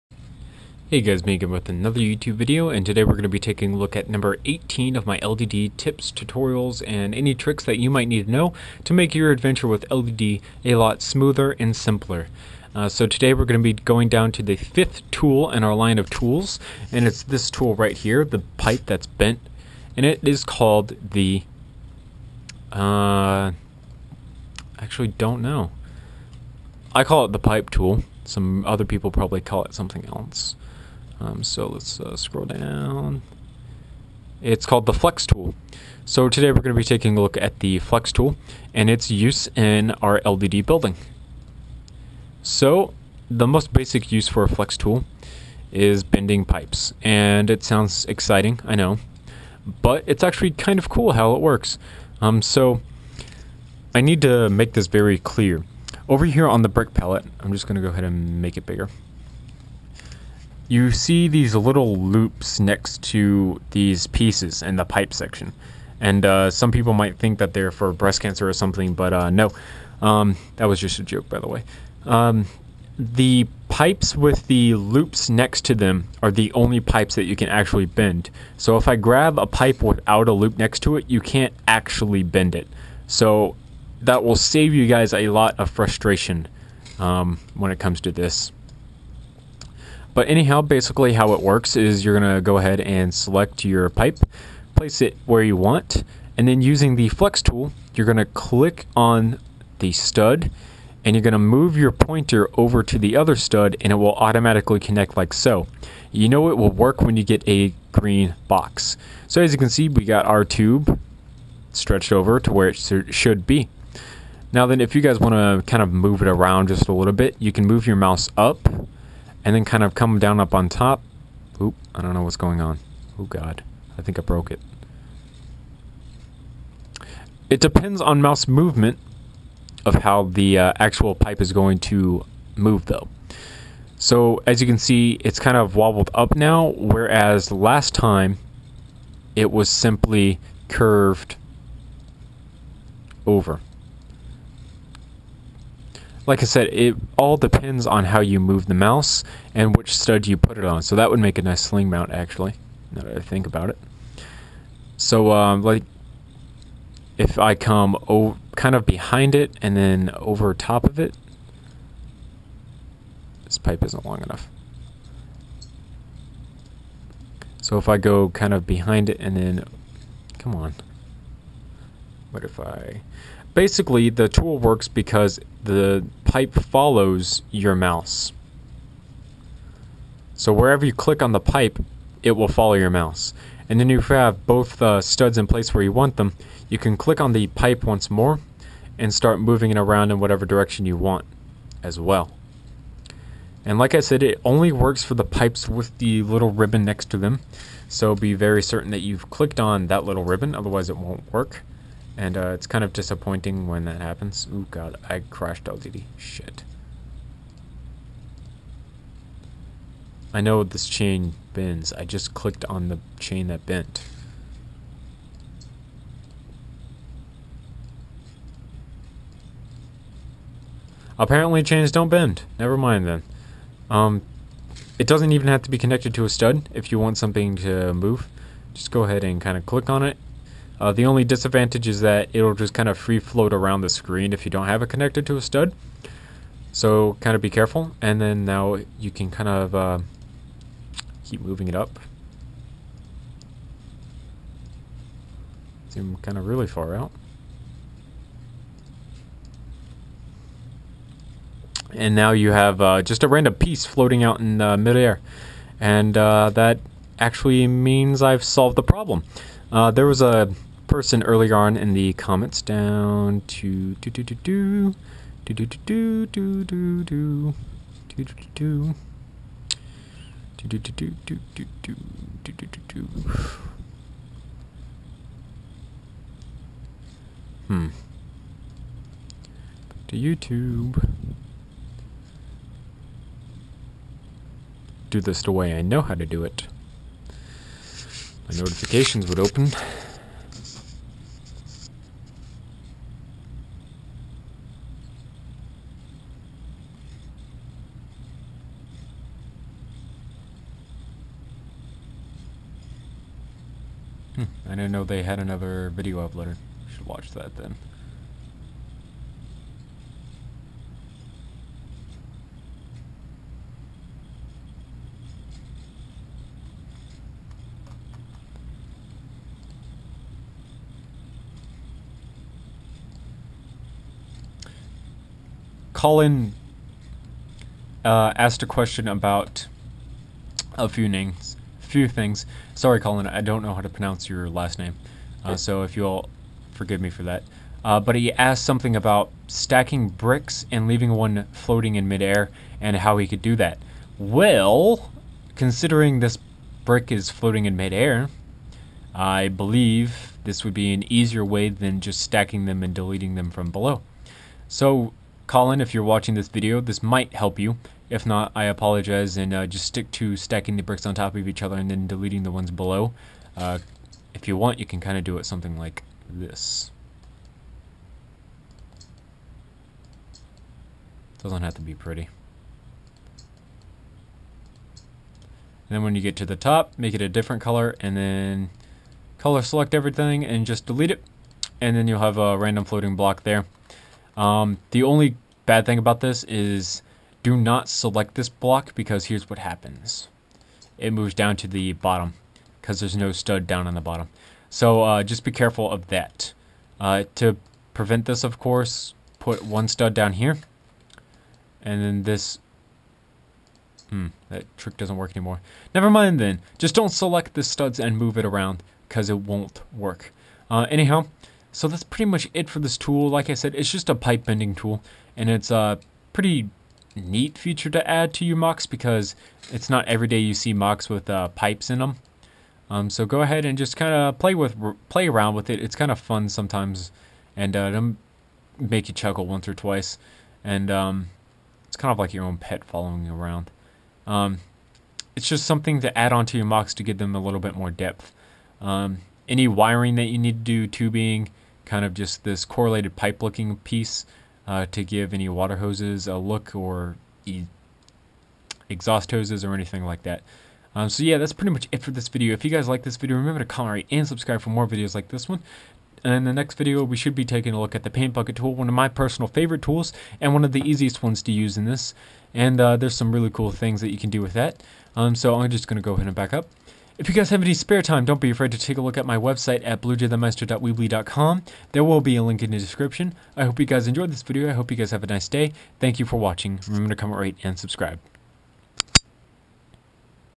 Lego Digital Designer; Tutorial, Tips, Tricks: #18 Flex Tool
Here is some help about the Flex Tool in LDD.